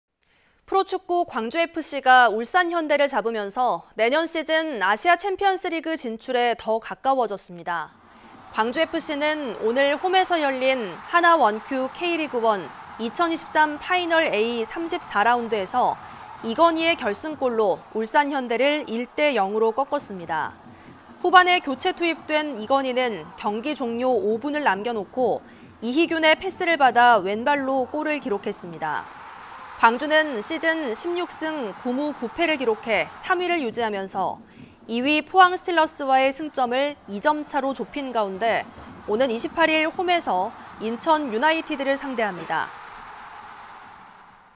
'돌풍' 광주FC, 울산 잡고 ACL 성큼
프로축구 광주FC가 울산현대를 잡으면서
내년 시즌 아시아챔피언스리그 진출에 더 가까워졌습니다.

광주FC는 오늘 홈에서 열린
하나원큐 K리그1 2023 파이널A 34라운드에서
이건희의 결승골로 울산 현대를 1-0으로 꺾었습니다.

후반에 교체투입된 이건희는 경기 종료 5분을 남겨놓고,
이희균의 패스를 받아 왼발로 골을 기록했습니다.

광주는 시즌 16승 9무 9패를 기록, 3위를 유지하면서
2위 포항 스틸러스와의 승점을 2점차로 좁힌 가운데
오는 28일 홈에서 인천 유나이티드를 상대합니다.